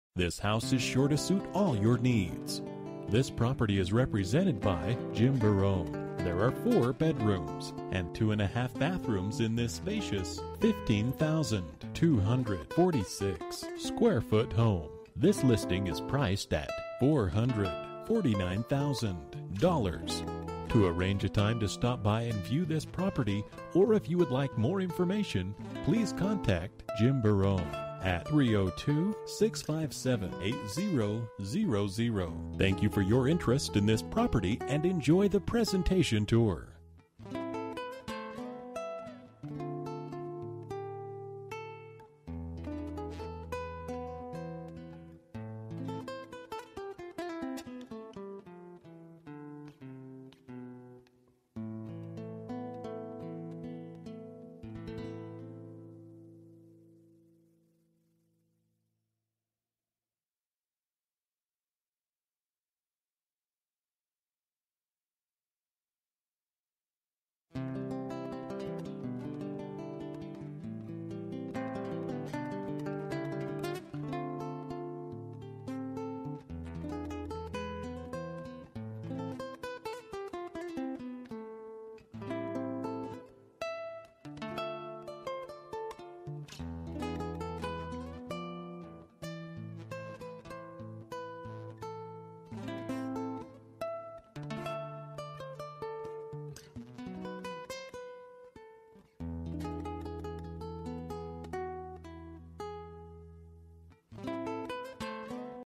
18 BALMORAL DR
Bedrooms: 4
Bathrooms: 2.5 bathrooms
Square Feet: 15246
Price: $449,000

03/18/2014 09:52:05 am

Last modified: